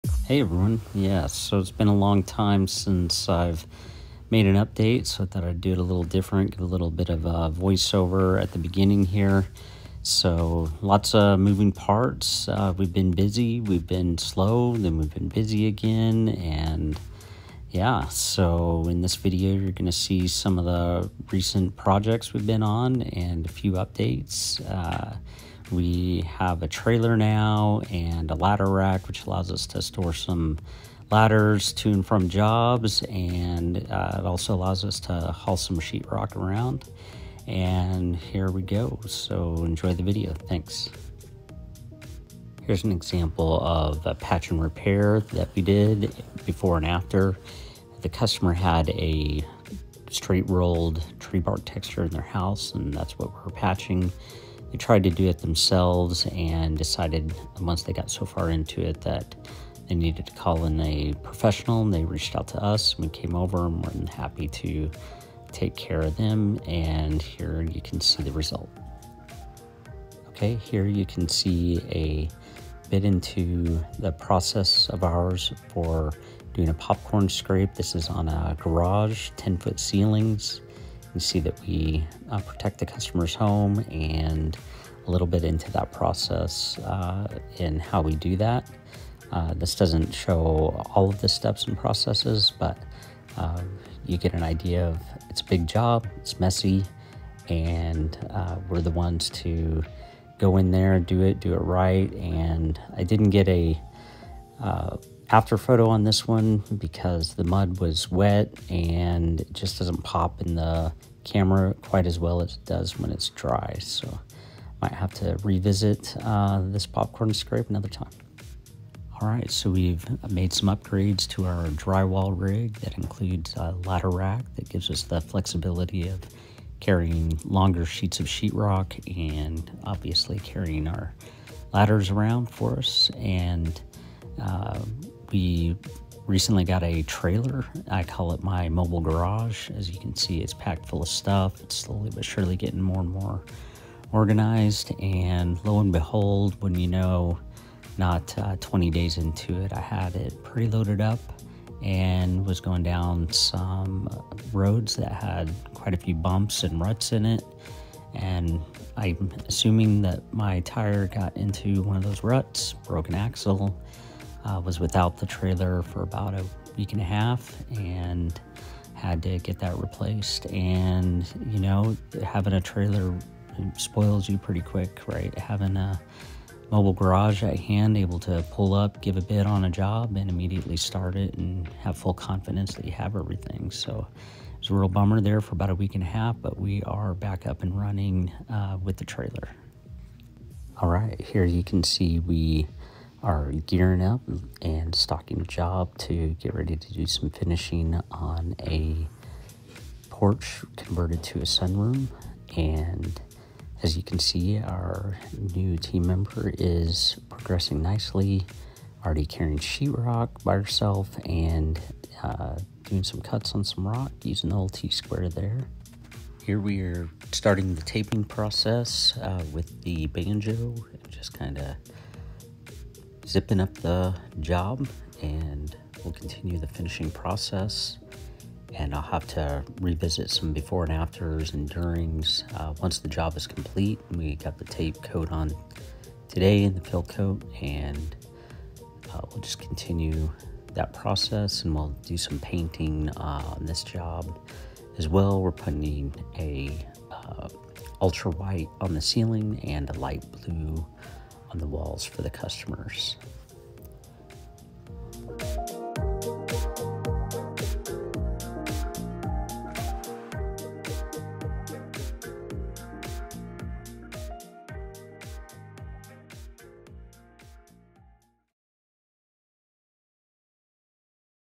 Drywall repairs. Sunroom room addition. Taping with banjo drywaller construction Springfield 65807
It has been a while since I have posted. It has been busy then slow then busy again!  This video shows a few jobs as well as some updates we made to our primary drywall rig.

I did this one a little different and did a voiceover throughout.